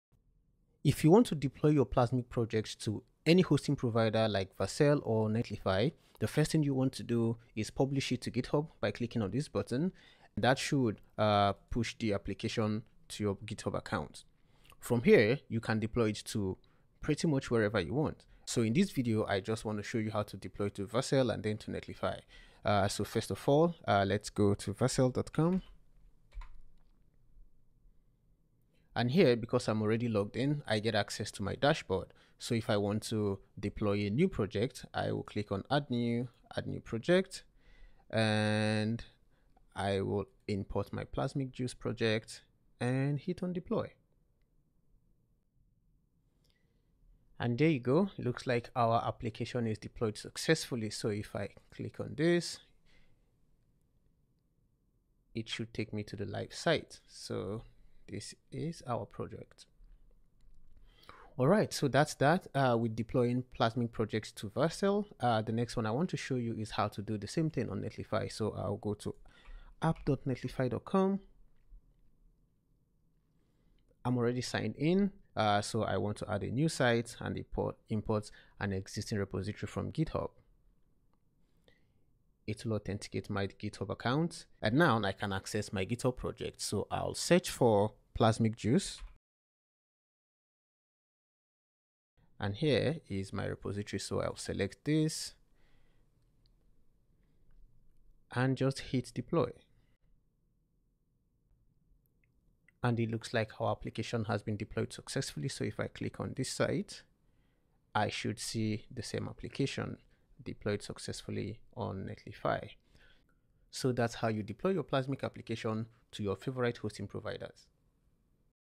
Deploy Plasmic Project to Netlify and Vercel
Learn how to deploy Plasmic projects to both Netlify and Vercel in 2 minutes

Transcript:
If you want to deploy your Plasmic projects to any hosting provider like Vercel or Netlify the first thing you want to do is publish it to GitHub by clicking on this button that should, uh, push the application to your GitHub account. From here, you can deploy it to pretty much wherever you want. So in this video, I just wanna show you how to deploy to Vercel and the Netlify.

Uh, so first of all, uh, let's go to vercel.com.

And here because I'm already logged in. I get access to my dashboard. So if I want to deploy a new project, I will click on add new, add new project and I will import my plasmic-juice project and hit on deploy.

And there you go. Looks like our application is deployed successfully. So if I click on this. It should take me to the live site. So this is our project. All right. So that's how we deploy plasmic projects to Vercel. Uh, the next one I want to show you is how to do the same thing, on Netlify. So I will go to netlify.com.

I'm already signed in. Uh, so I want to add a new site and import existing repository from Github. It'll authenticate my Github account and now I can access my Github project. So I'll search for plasmic juice

and here is my repository. So I'll select this

and just hit deploy.

And it looks like our application has been deployed successfully. So if I click on this site, I should see the same application. Deployed successfully on Netlify. So that's how you deploy your plasmic application to your favourite hosting providers.